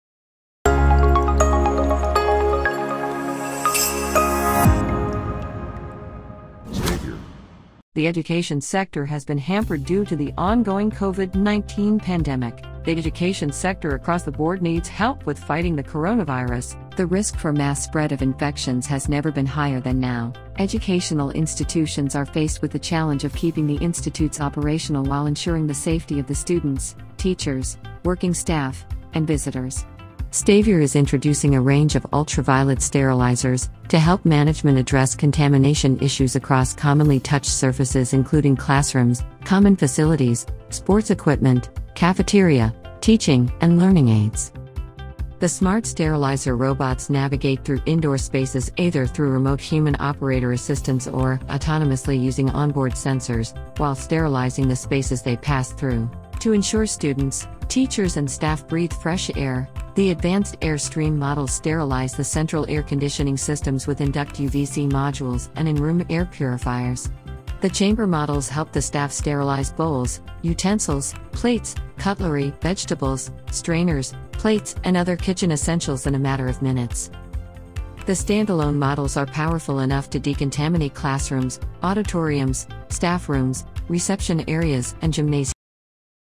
Disinfect your schools area with UV Sterilizers. Keep your students safe.
Know  how UV lights/sterilizers disinfect the school's area

All commonly touched surfaces including classrooms, common facilities, sports equipment, cafeteria, teaching, and learning aids must be sterilized daily.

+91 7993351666